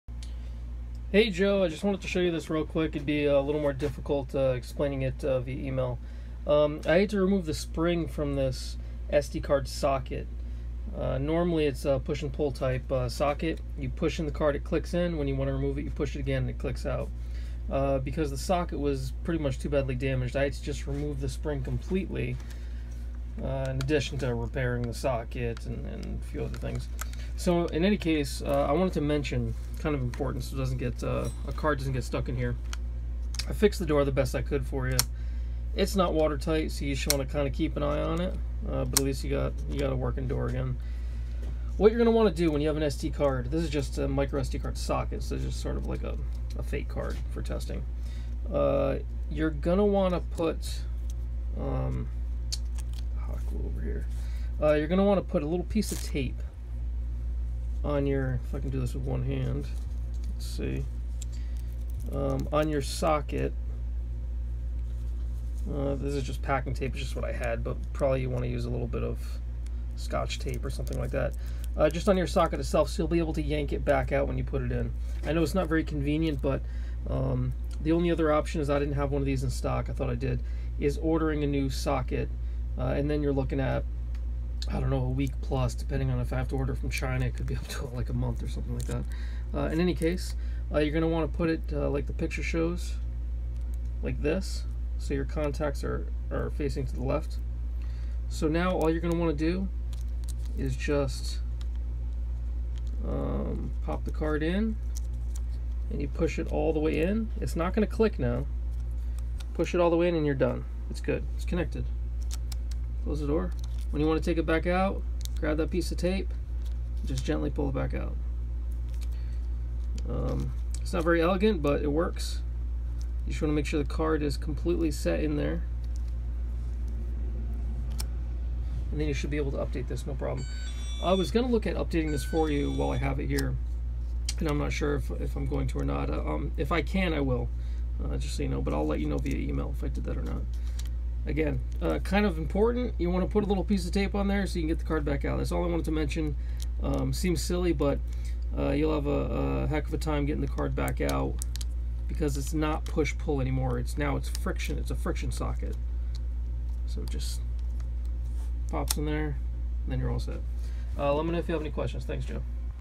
Garmin 740s chartplotter - SD Socket Repair
Repaired Garmin 740s chartplotter with a defective SD card socket.  In order to expedite the repair, the card socket itself was repaired.  Due to damage we had to also convert the socket from push-pull to friction retention.

Simple instruction to customer to put a small piece of scotch tape on card when inserting into socket in order to make removal easier and prevent future damage.